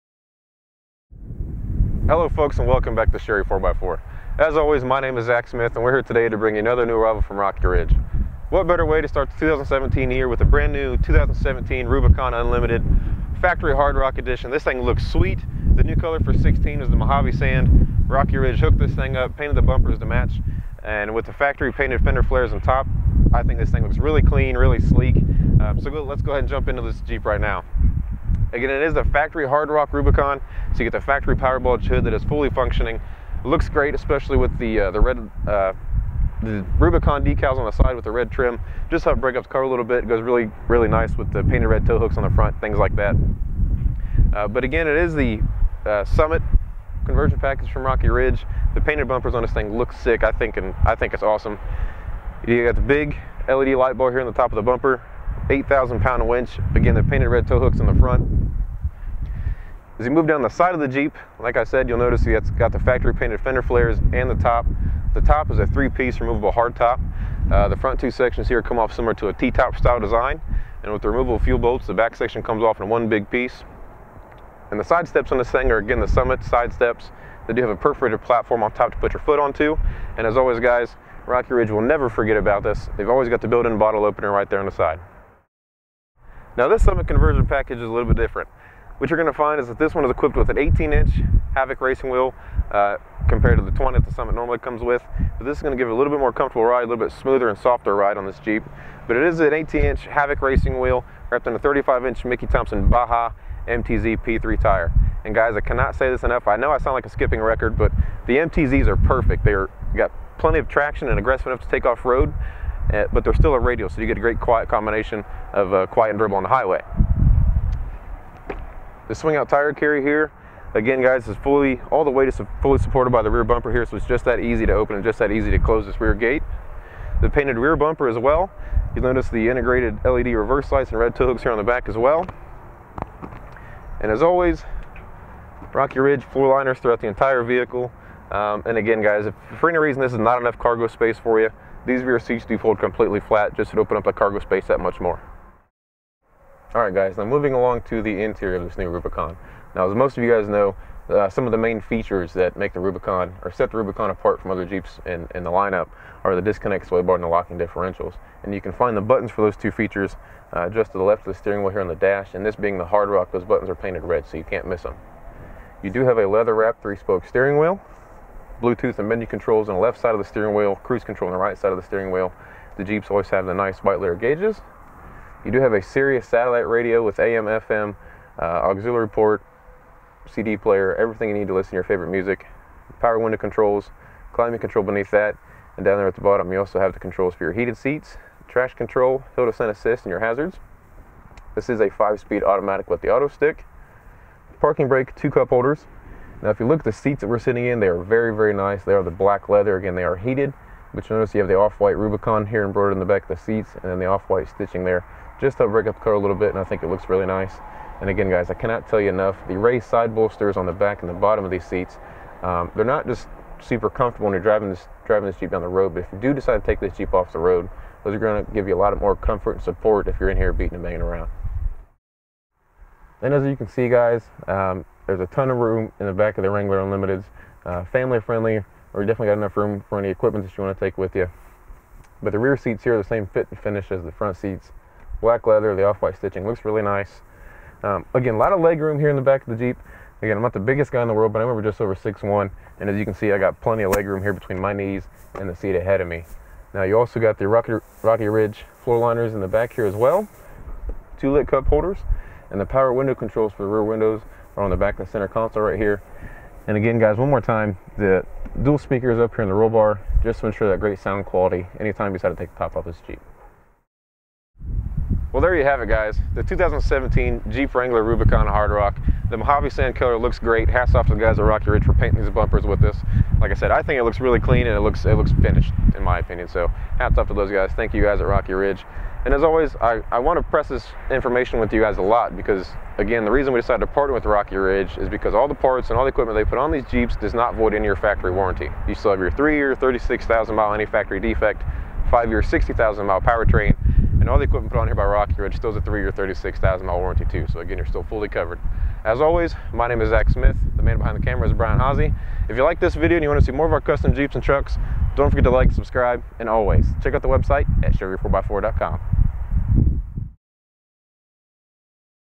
2017 Jeep Wrangler Unlimited Rubicon Hard Rock - Rocky Ridge K2 In-Depth Walkthrough | 27818T
Zac Smith of Sherry4X4 walks you through this wild 2016 Jeep Wrangler Unlimited Rubicon that has been that has been upgraded with Rocky Ridge's brand new Mad Rock package!

» Stock Number: 27818T

This Rocky Ridge K2 Conversion Includes:

3.5″ Rocky Ridge Sport Suspension Lift
Performance Coil Springs
Nitrogen Gas-Charged Shocks
High Steer Conversion
Replacement Front and Rear Sway-Bar End Links
Geometry Correction Bracket
Foam Bump Stops
Brake-Line Adapter Bracket
Adjustable Front & Rear Track Bars
Upgraded Sway Bar
18″ Black Powder Coated Wheels
35″ Mickey Thompson Tire Baja MTZ Tires
Painted Rocky Ridge Summit Front Bumper
Painted Rocky Ridge Summit Rear Bumper
Double Hinged Swing Out Tire Carrier
Off Road Running Boards
8,500 lb Synthetic Rope Winch
LED Off Road Spot Lights
LED Back-Up Lights
Nylon Web Roll Bar Straps w/ Molded Grip Handles
Rugged Ridge All Weather Floor & Cargo Liners
Front and Rear Light Grilles
Speedometer Calibration Module
3 Year/36,000 Mile Warranty On All Accessories
Road Force Balanced Tires
4 Wheel Alignment